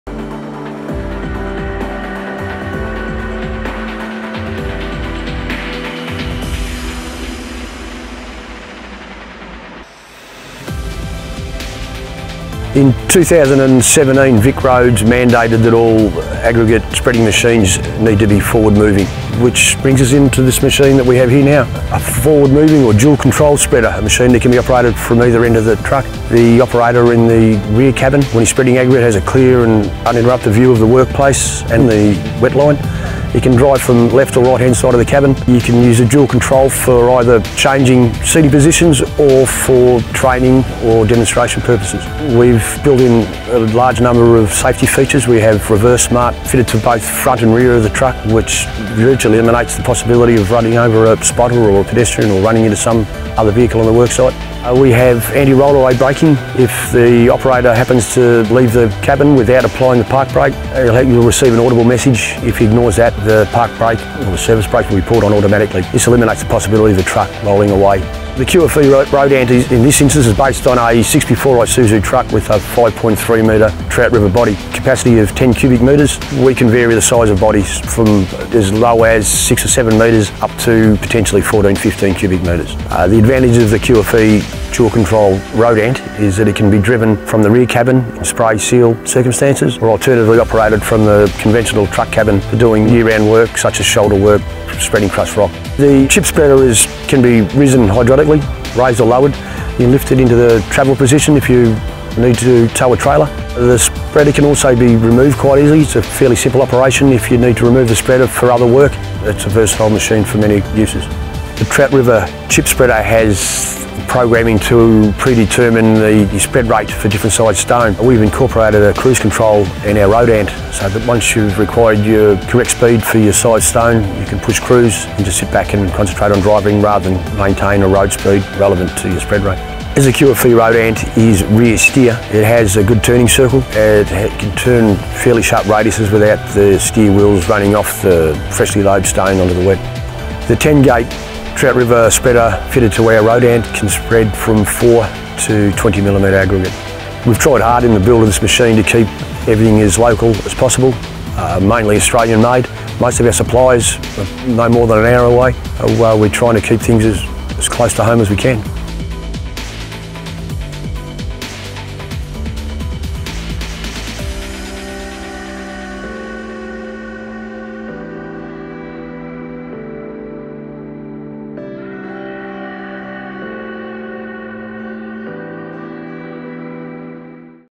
Q-FE ROAD ANT Forward Moving Aggregate Spreader
The Q-FE Road Ant is an Australian designed and manufactured forward moving aggregate spreader. It is unique in that it has dual controls, meaning that the aggregate spreader can be driven in a forward motion from both the front and the rear cabin whilst spreading aggregate.  It is a versatile machine that can be used 365 days a year by changing attachments.

For more information please visit @qualityfab.com.au